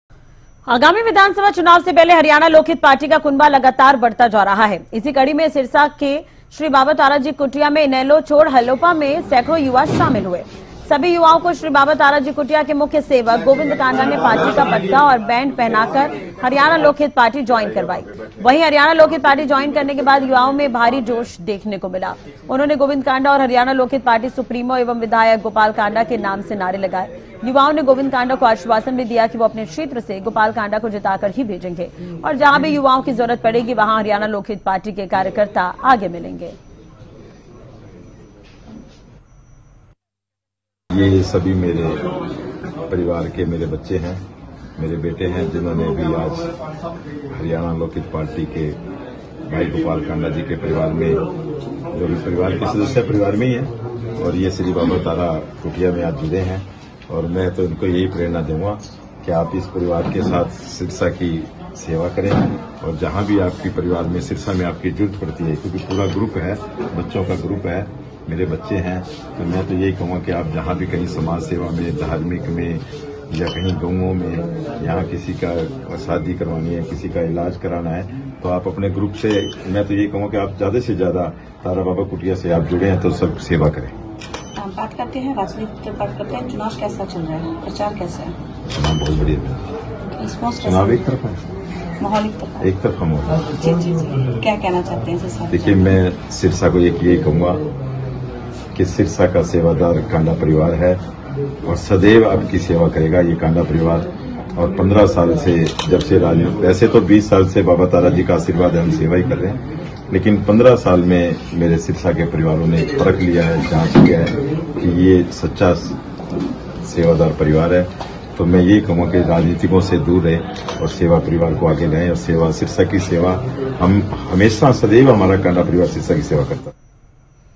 चुनाव से पहले हरियाणा लोकहित पार्टी का बढ़ा कुनबा, इनेलो छोड़ हलोपा में शामिल हुए सैंकड़ों युवा
आगामी विधानसभा चुनाव से पहले हरियाणा लोकहित पार्टी का कुनबा लगातार बढ़ता जा रहा है. इस कड़ी में सिरसा की श्री बाबा तारा जी कुटिया में इनेलो छोड़ हलोपा में सैंकड़ों युवा शामिल हुए.